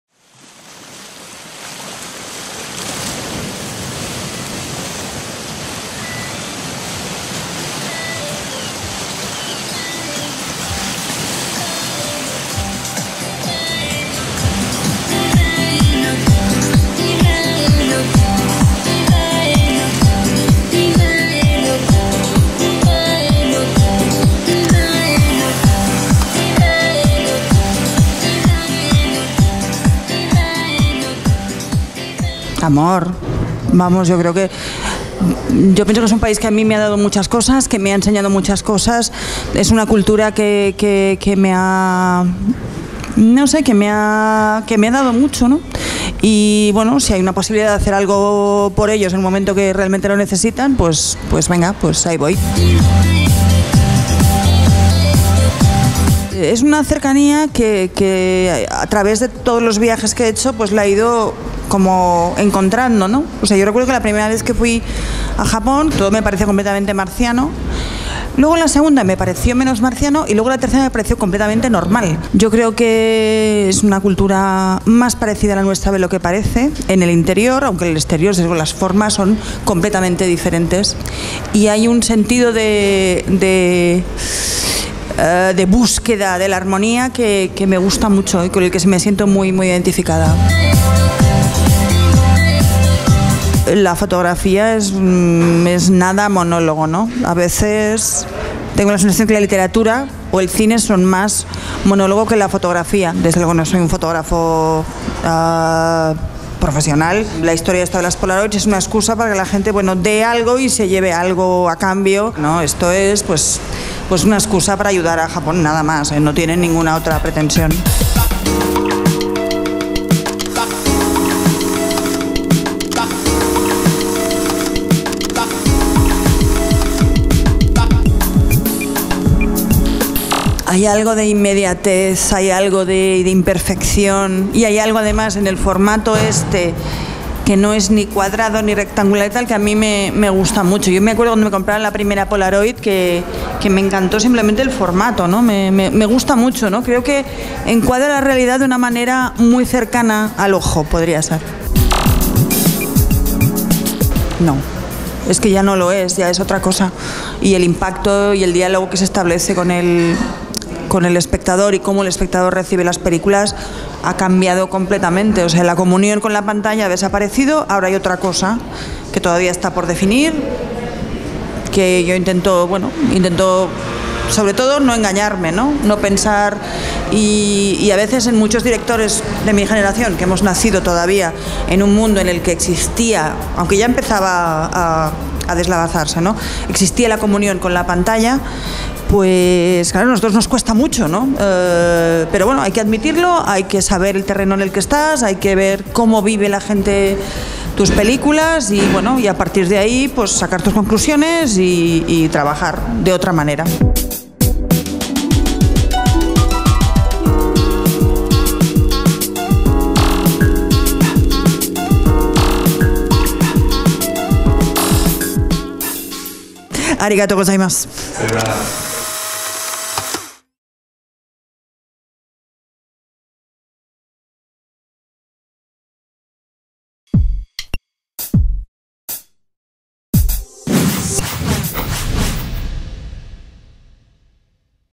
Isabel Coixet solidaridad con Japón
Entrevista realizada a la directora de cine Isabel Coixet en la presentación de su exposición de fotografías "Polaroids for Japan", en la galería Raíña Lupa de Barcelona. Con el objetivo de recaudar fondos para la construcción de una escuela en las localidades debastadas por el tsunami de Japón.Vídeo para